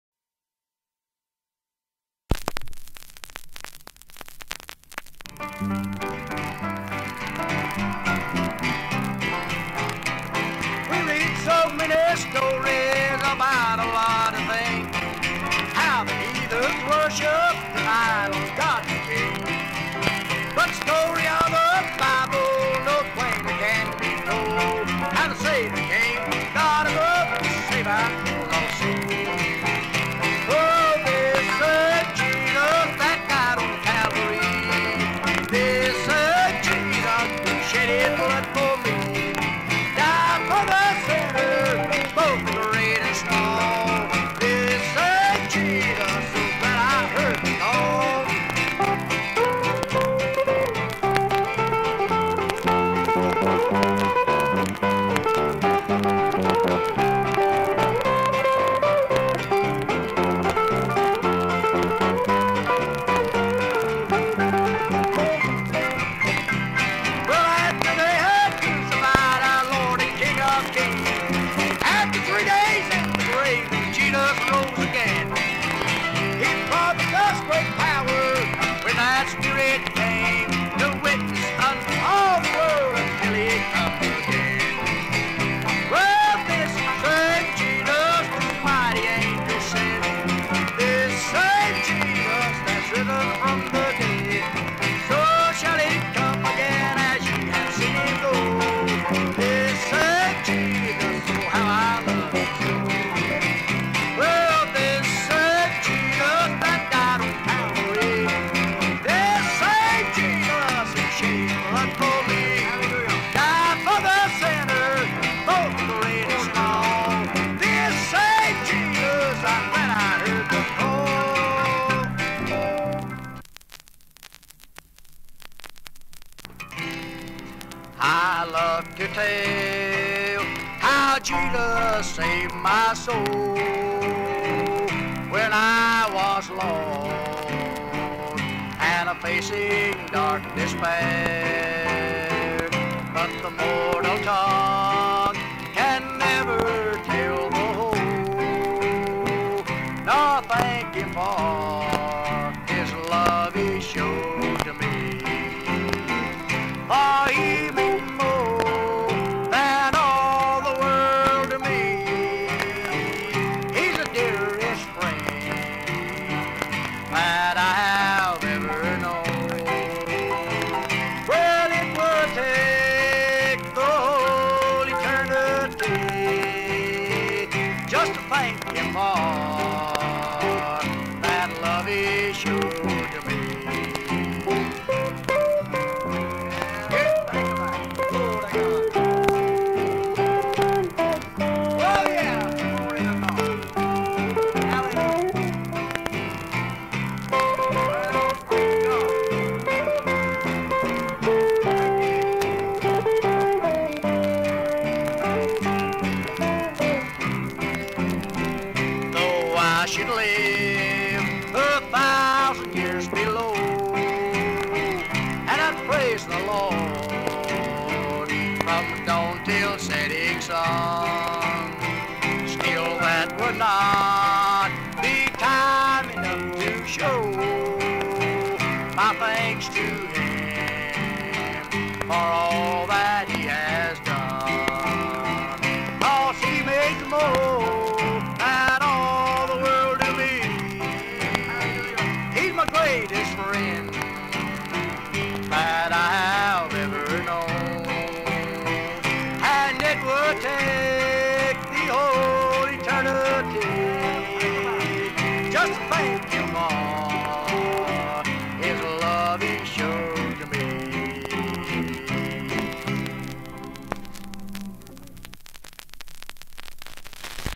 Happy Hatley Side 1
Very lively sanctified music from Greenville, SC's Happy Hatley.